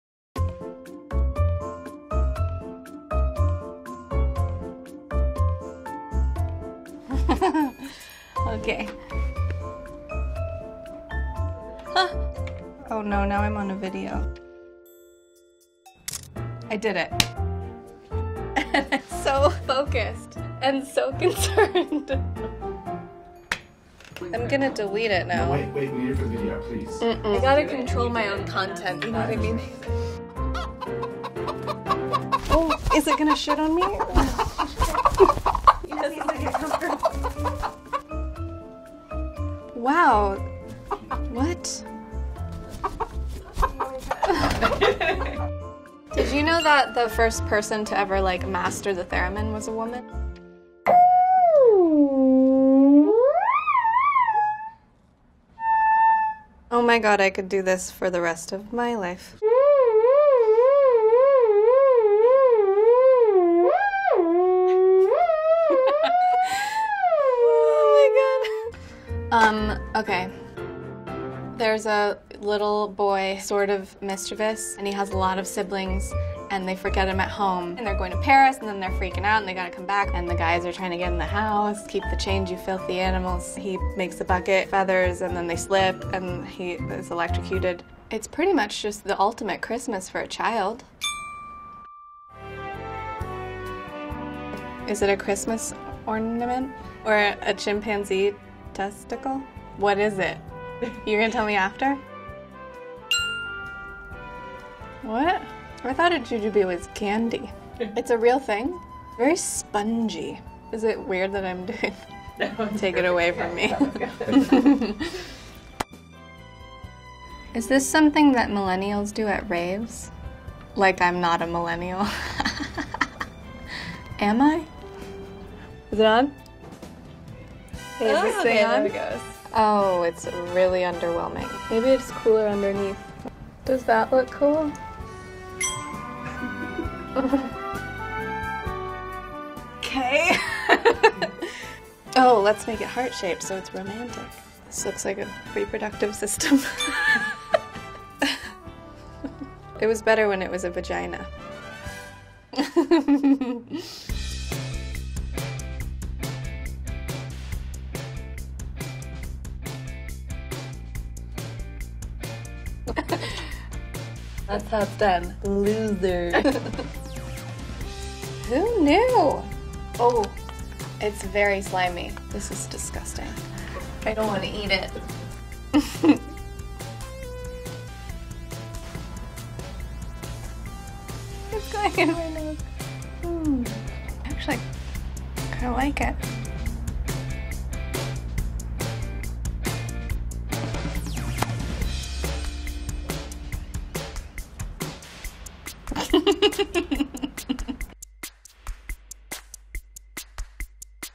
用腳拍的自拍不滿意 達珂塔·強生：可以把照片刪掉嗎？｜明星挑戰9件事｜Vogue Taiwan
有人猜到達珂塔·強生說的電影是哪一部嗎？？

【追蹤 VOGUE TAIWAN】
★LINE：VogueTaiwan